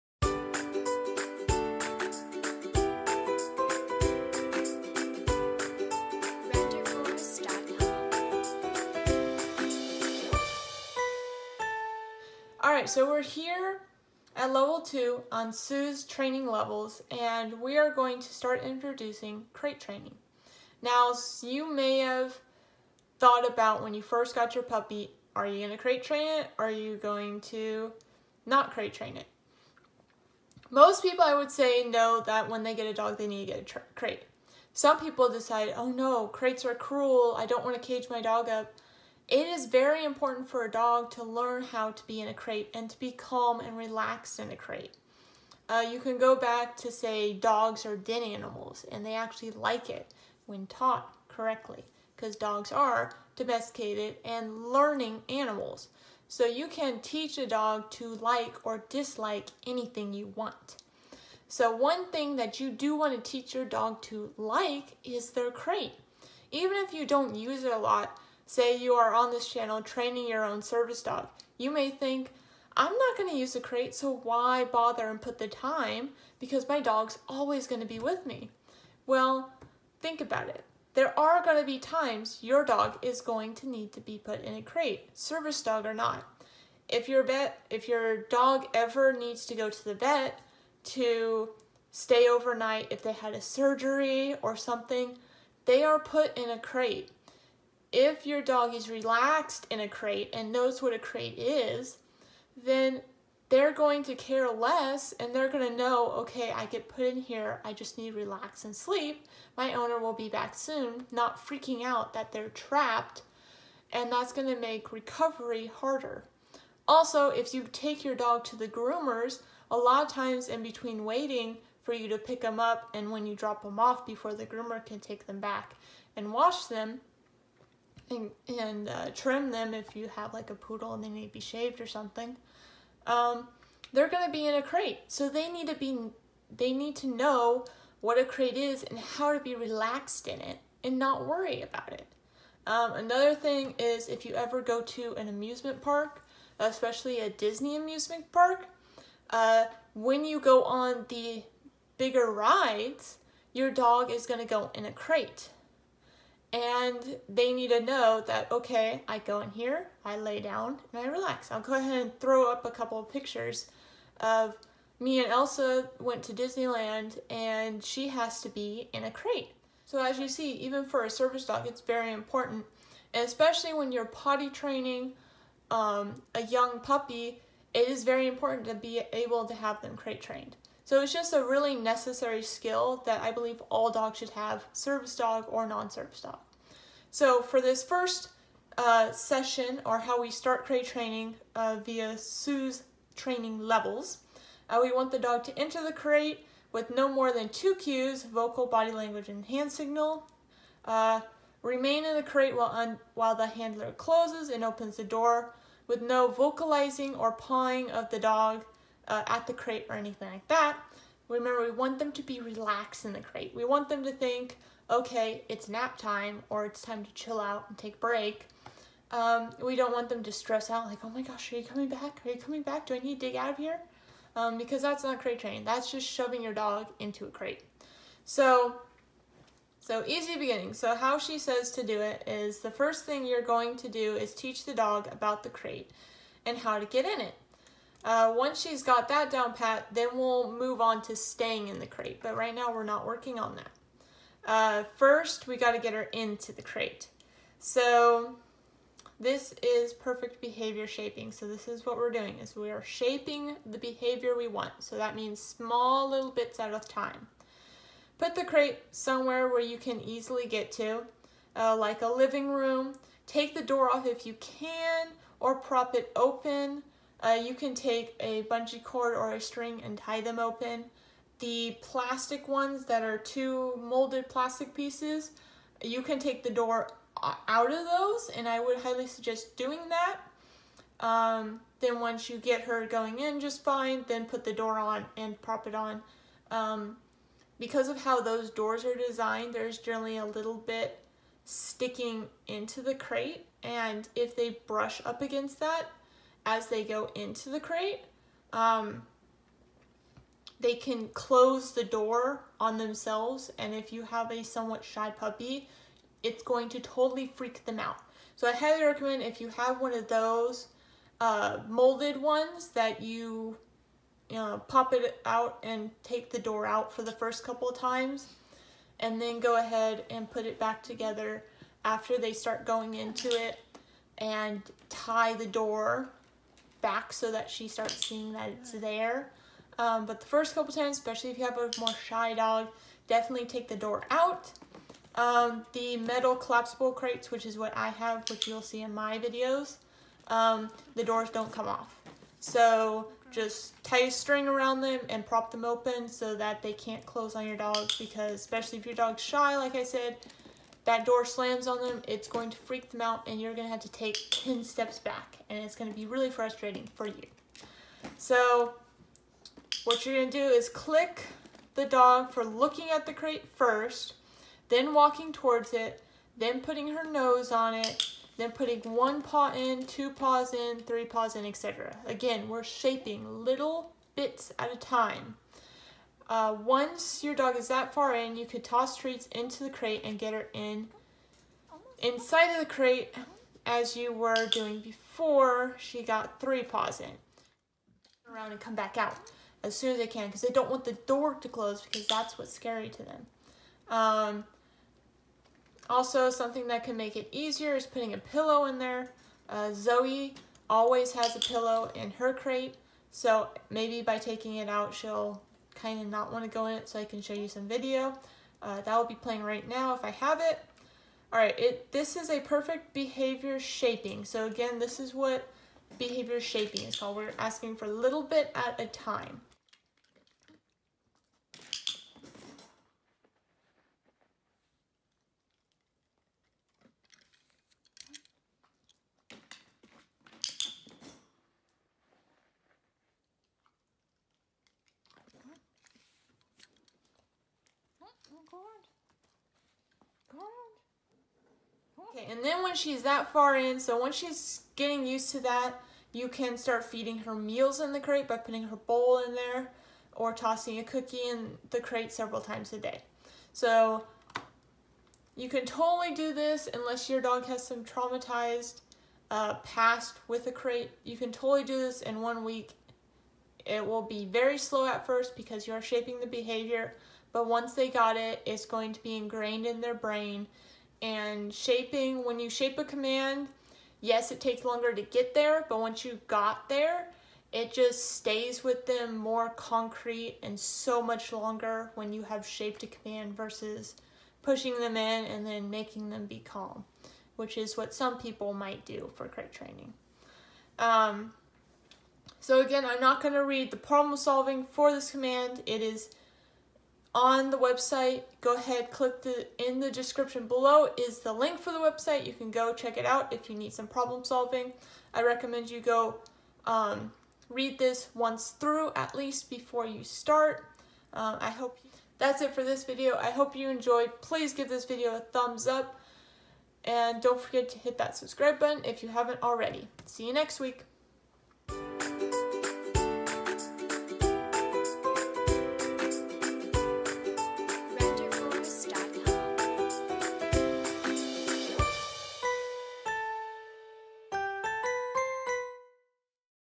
Level 2 "Crate" Sue Ailsby Training Levels Vlog
This week we are starting crate training. One of the most understated parts to any dog's training.